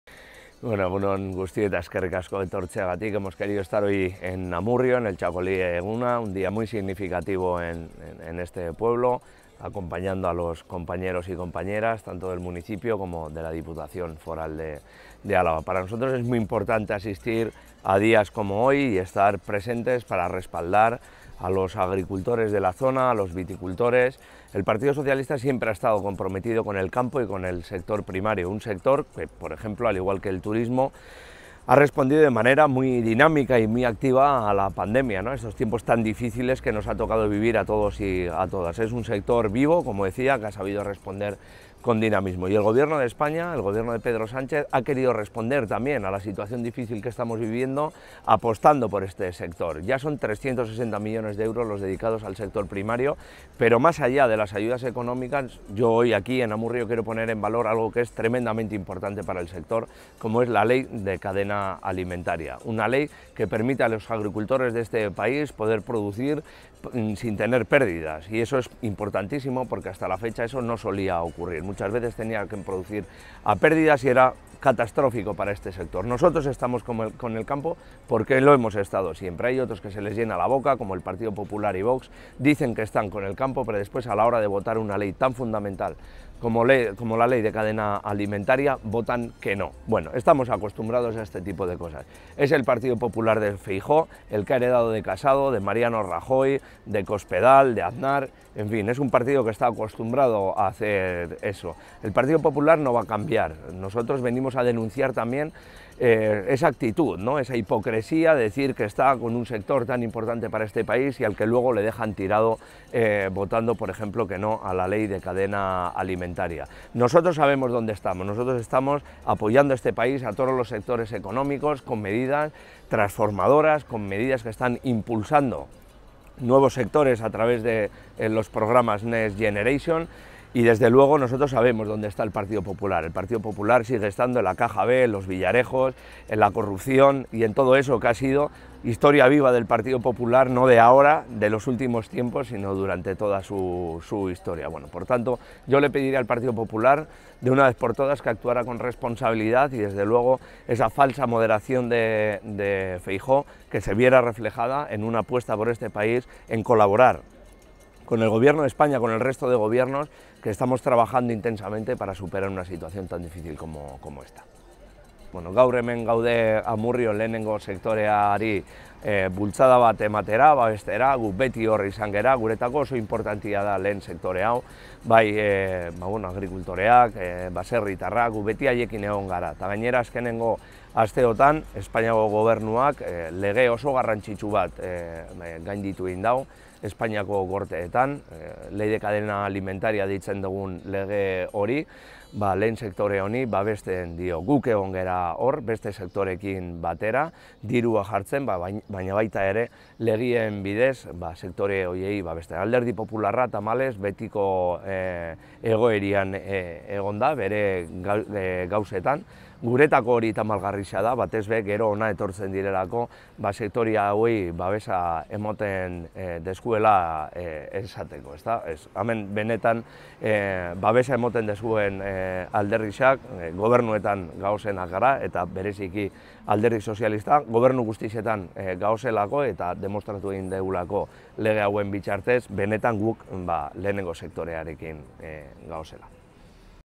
El Secretario General del PSE-EE, Eneko Andueza, participa en el XXIII Txakolin Eguna de Amurrio
Eneko Andueza: “Los Socialistas estamos con el campo, el PP lo ha dejado tirado porque está en la caja B, los Villarejos y la corrupción”
El Secretario General del PSE-EE, que ha participado en el XXIII Txakolin Eguna de Amurrio, reprocha a PP y Vox que votaran en contra de la Ley de Cadena Alimentaria